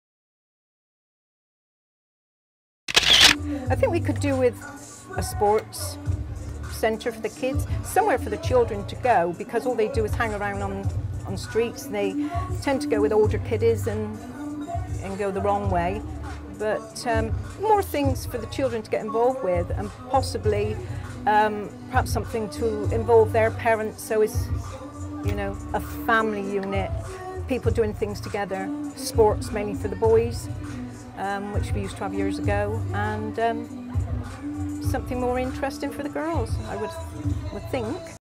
What would make Knowle a better place to live - 3
Work from the Futures Conference shown to local people at the Filwood Funday on 19th September. Exhibition to communicate and explain the Knowle West objectives and vision to the community, and to test its support with them.
Knowle West Media Centre had a video booth and local people could come in and say what they think about the future of Knowle West.